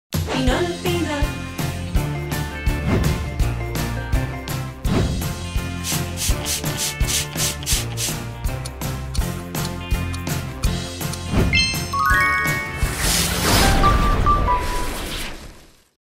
Desodoriza y desinfecta las superficies de casa con Pinol® El Original. 🌲
Atomiza el área deseada con Pinol® El Original para desodorizar las superficies, después enjuaga con un poco de agua y listo. ¡El poder de Pinol® El Original todo lo puede! Con cualquiera de sus 3 aromas Pino, Cítrico y Floral. 💪🏼